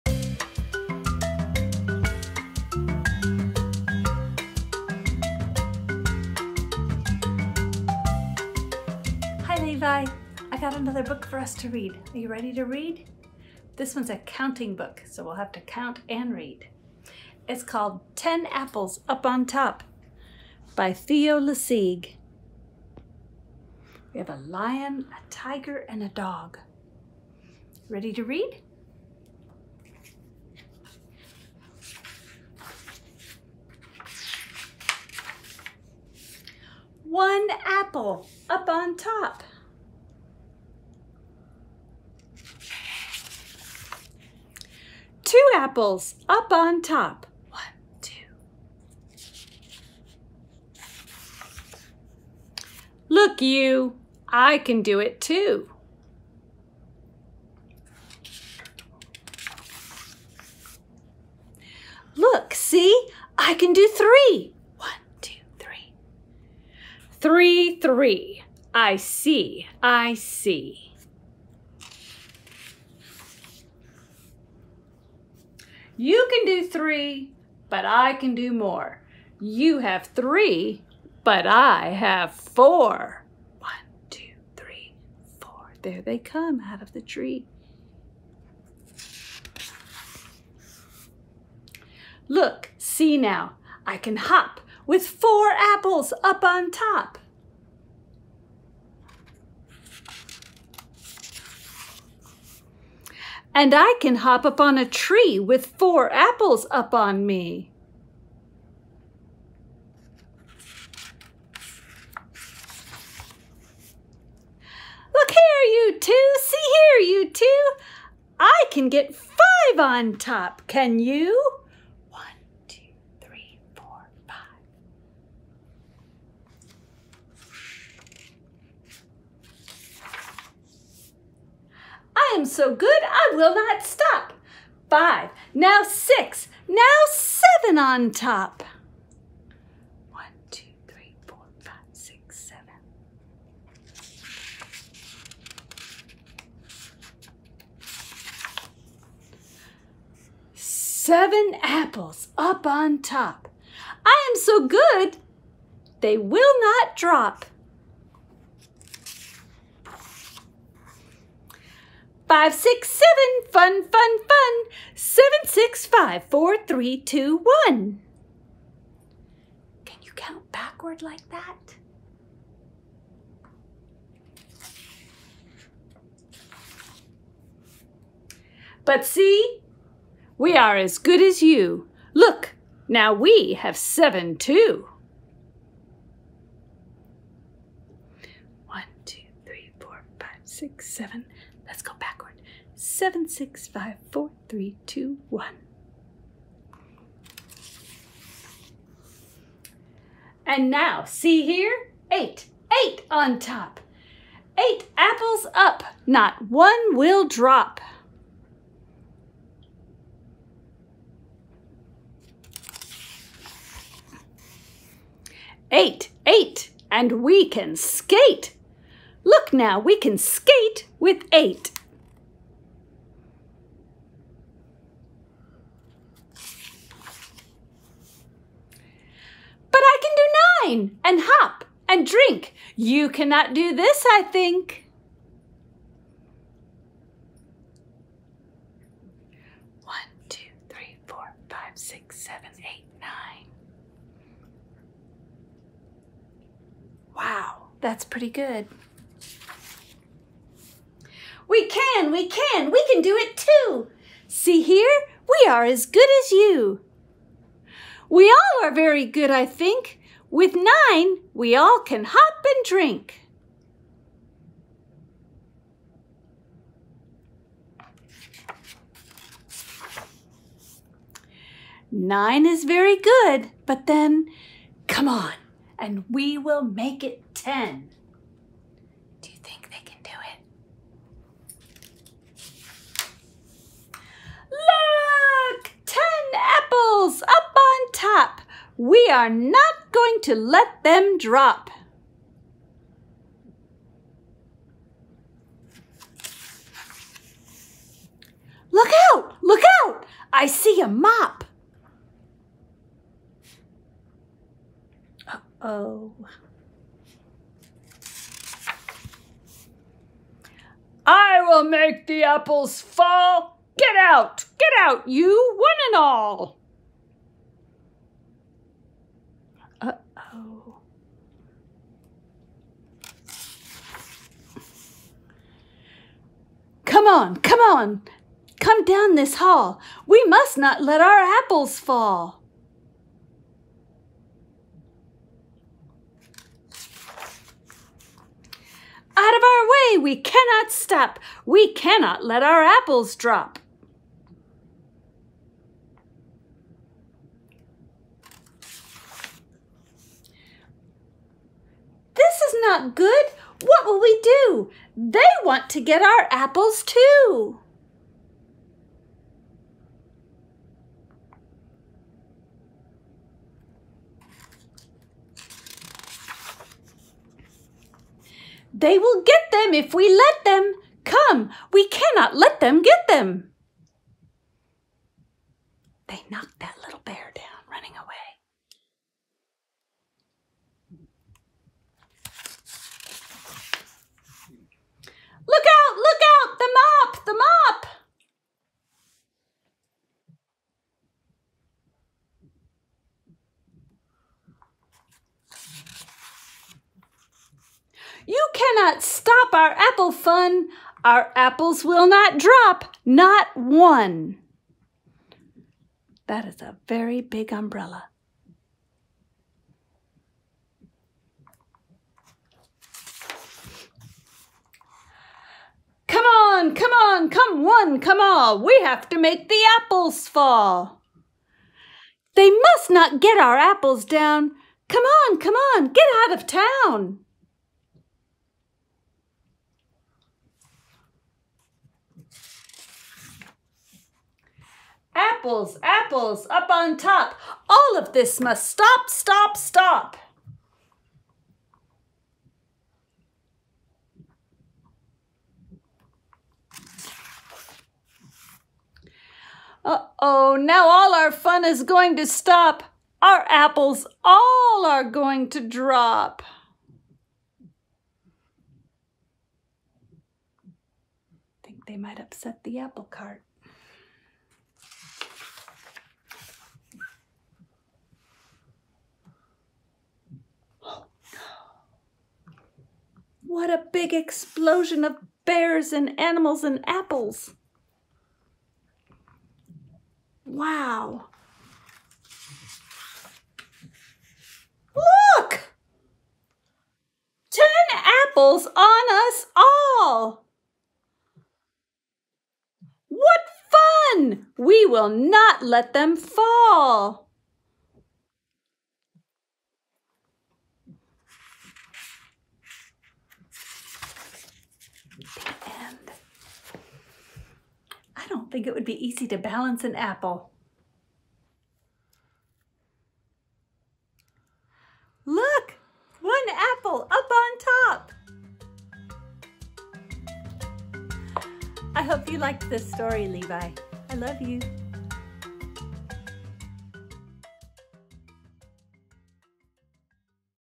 Ten Apples Up On Top - Kids Book Read Aloud
Kiri reads the Theo. LeSieg (Dr. Seuss) classic counting book illustrated by Roy McKie.